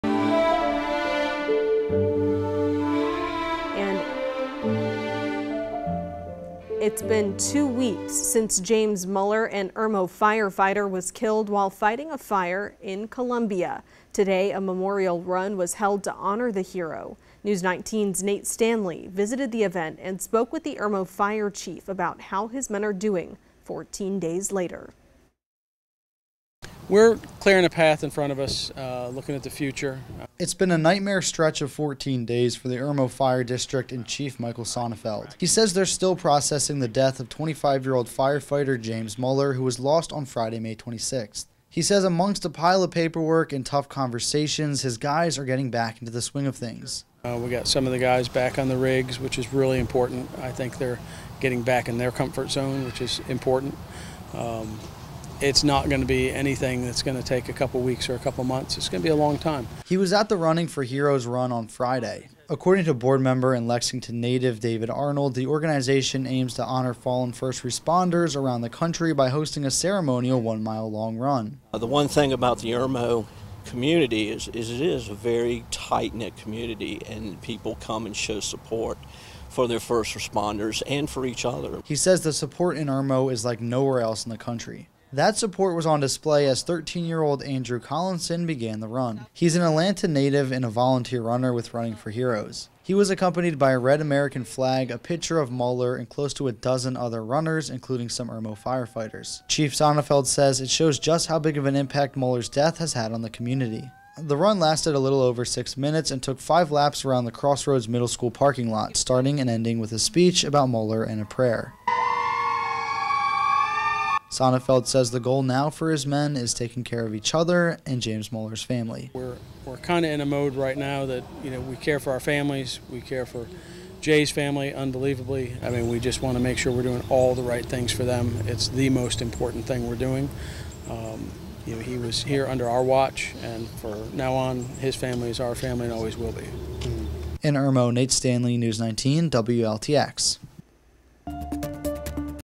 Running 4 Heroes honors fallen Irmo firefighter James Muller
Muller died while fighting a fire at the Tropical Ridge apartment complex.